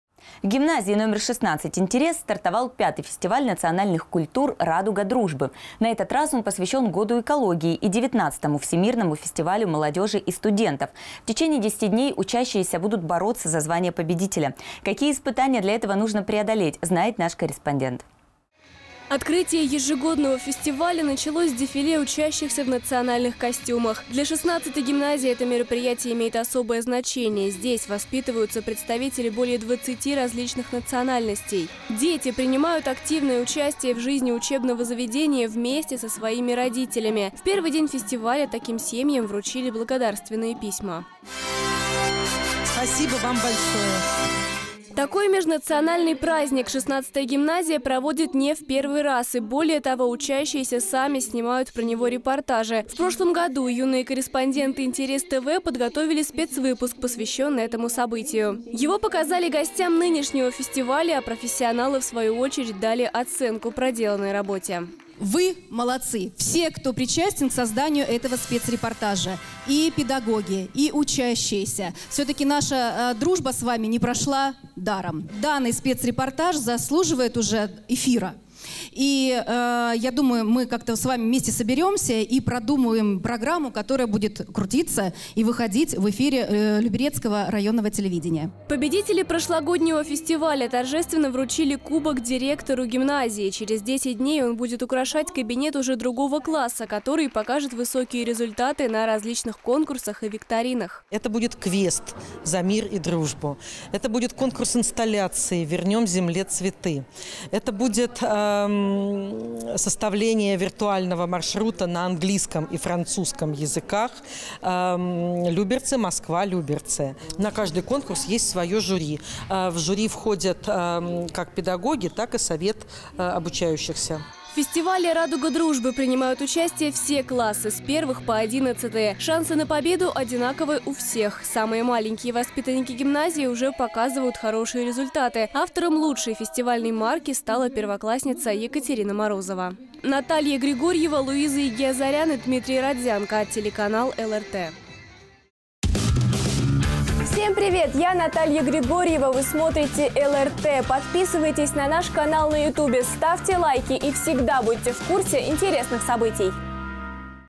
Стартовал пятый фестиваль национальных культур «Радуга дружбы»
В гимназии No16 «Интерес» стартовал пятый фестиваль национальных культур «Радуга дружбы». На этот раз он посвящен году экологии и 19-ому Всемирному фестивалю молодежи и студентов. В течение 10 дней учащиеся будут бороться за звание победителя. Какие испытания для этого нужно преодолеть – знает наш корреспондент.

Открытие ежегодного фестиваля началось с дефиле учащихся в национальных костюмах. Для 16-ой гимназии это мероприятие имеет особое значение, здесь воспитываются представители более 20 различных национальностей. Дети принимают активное участие в жизни учебного заведения вместе со своими родителями. В первый день фестиваля  таким семьям вручили благодарственные письма.

Такой межнациональный праздник 16-ая гимназия проводит не впервые, и, более того, учащиеся сами снимают про него  репортажи. В прошлом году юные корреспонденты «Интерес ТВ» подготовили спецвыпуск, посвященный этому событию. Его показали гостям нынешнего фестиваля, а профессионалы, в свою очередь, дали оценку проделанной работе.

Светлана Дембицкая, главный редактор телеканала «ЛРТ»:
"Вы-молодцы, все, кто причастен к созданию этого спецрепортажа: и педагоги, и учащиеся. Все-таки, наша дружба с вами не прошла даром. Данный спецрепортаж заслуживает эфира. Я думаю, мы с вами вместе соберемся и продумаем программу, которая будет крутиться и выходить в эфире Люберецкого районного телевидения".

Победители прошлогоднего фестиваля торжественно передали Кубок директору гимназии. Через 10 дней он будет украшать кабинет уже другого класса, который покажет высокие результаты на различных конкурсах и викторинах, предусмотренных в рамках мероприятия.

Ирина Снегирева, директор гимназии No16 «Интерес», г. Люберцы:
"Это будет квест «За мир и дружбу», это будет конкурс инсталляций «Вернем Земле цветы», это будет составление виртуального маршрута на английском и французском языках «Люберцы- Москва- Люберцы». На каждый конкурс есть свое жюри, в жюри входят как педагоги, так и Совет обучающихся".

В фестивале «Радуга дружбы» принимают участие все классы с 1- 11-ые. Шансы на победу одинаковы у всех. Самые маленькие воспитанники гимназии уже показывают хорошие результаты, автором лучшей фестивальной марки стала первоклассница Екатерина Морозова.